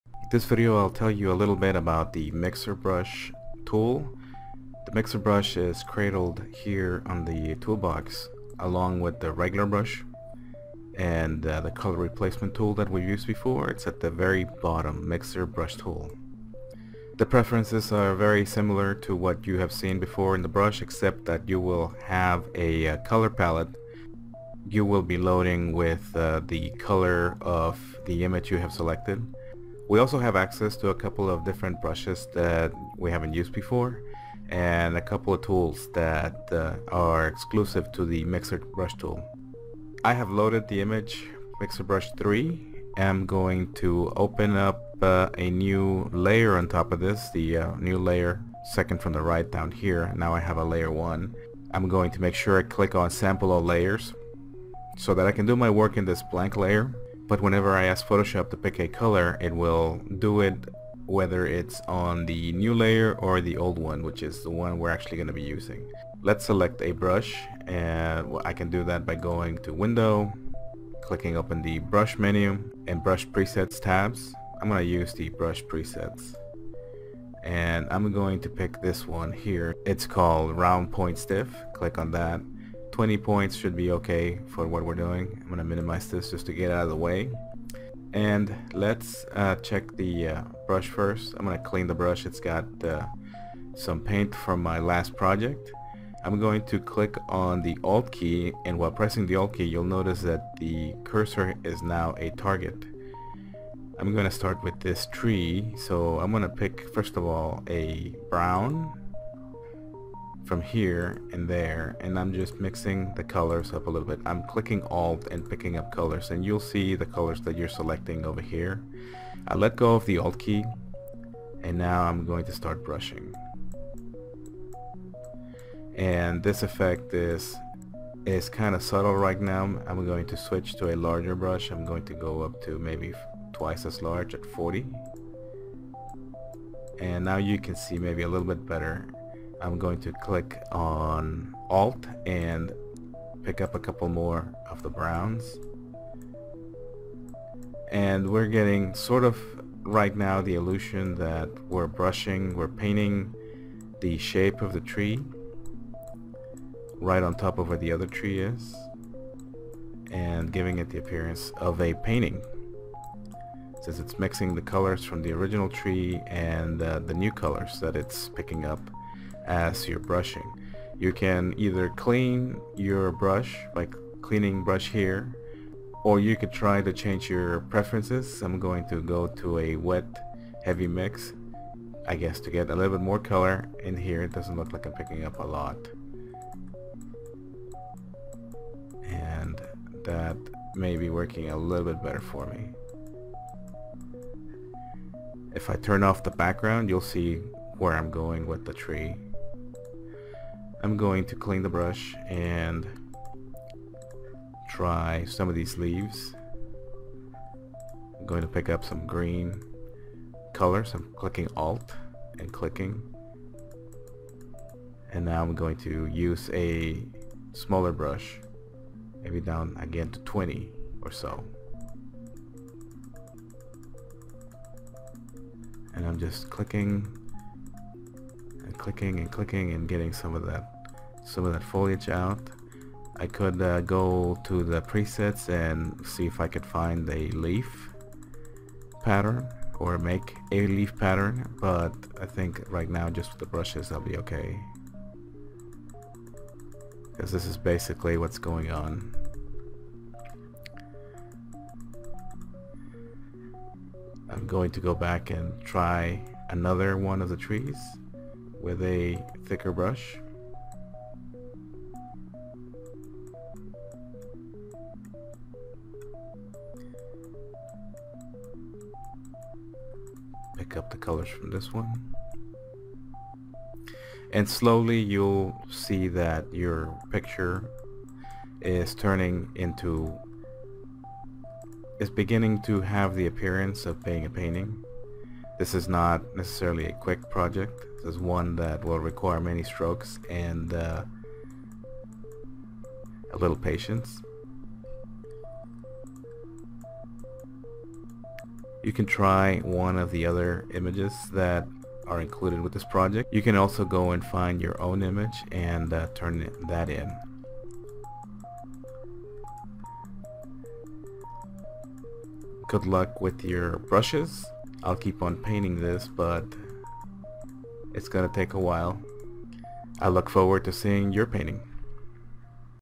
Use the Mixer Brush to Change a Photo
Quick run-through on how to load a brush and edit a photo using Photoshop's Mixer Brush tool.